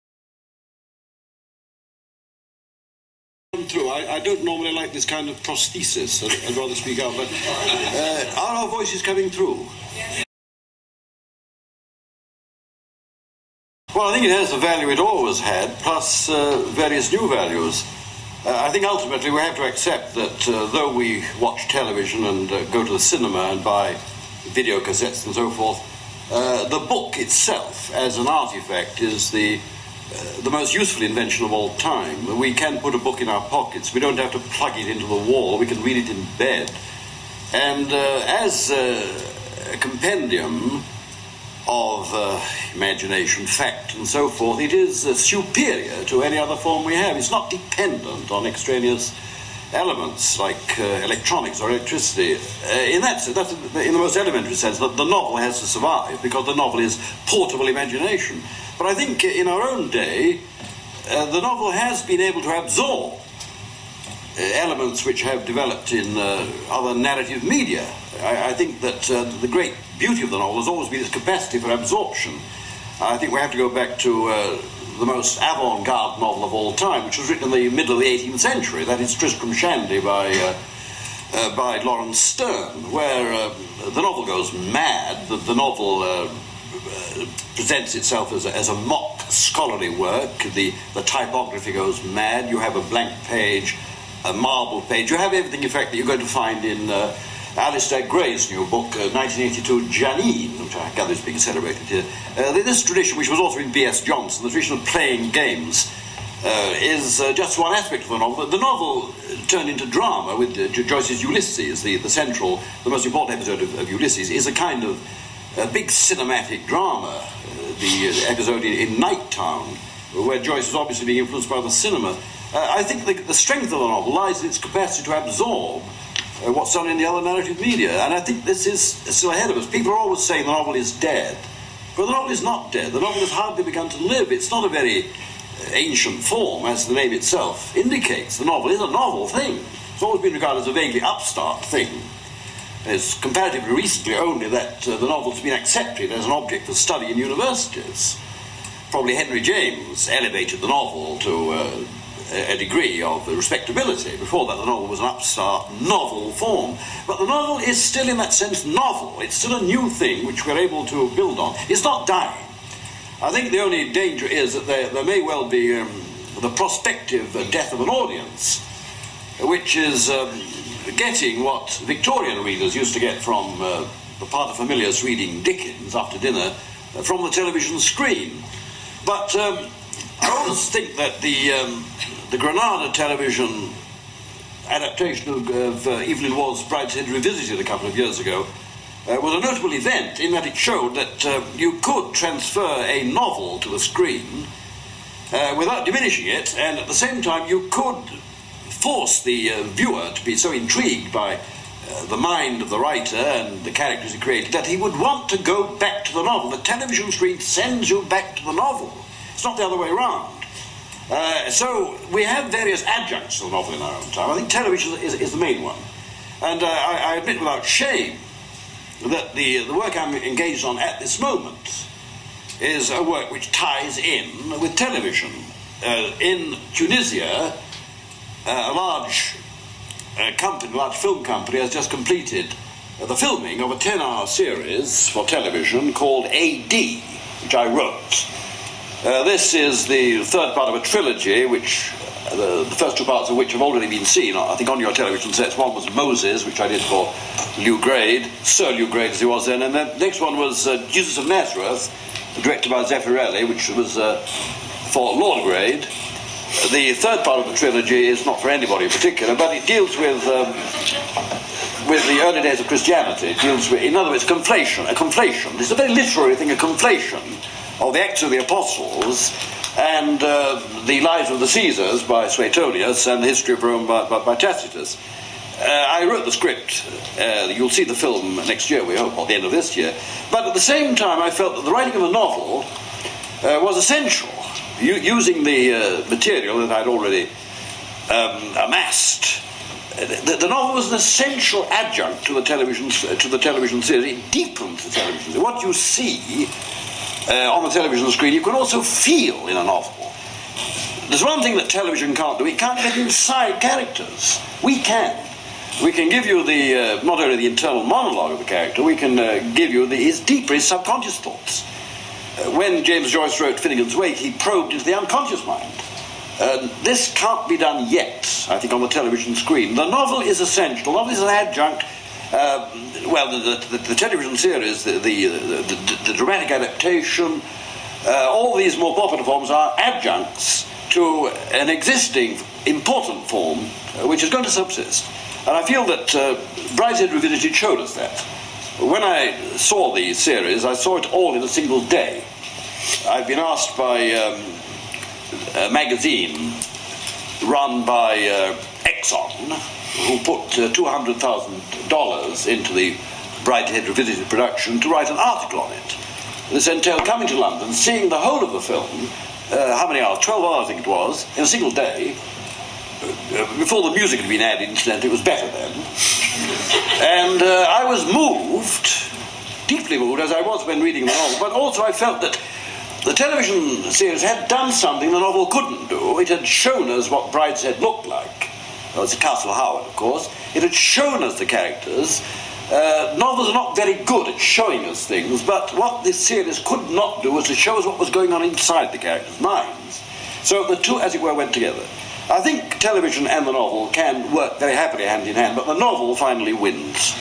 Anthony Burgess Speaks: 1984 — the novel today
Burgess on the continuing power and importance of the novel, with reference to Henry James, Dickens, Waugh, Joyce, Sterne, B.S. Johnson ('The future of the novel depends on people like B.S. Johnson,' Burgess wrote), Alasdair Gray and his own work, including television scripts.